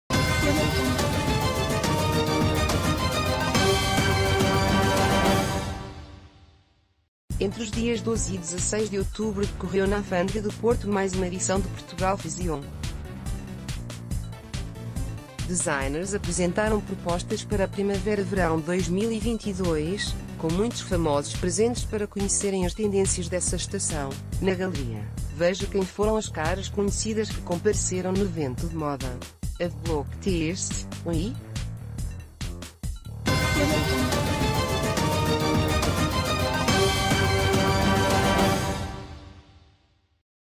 Caras | Famosos assistem aos desfiles do Portugal Fashion
Entre os dias 12 e 16 de outub...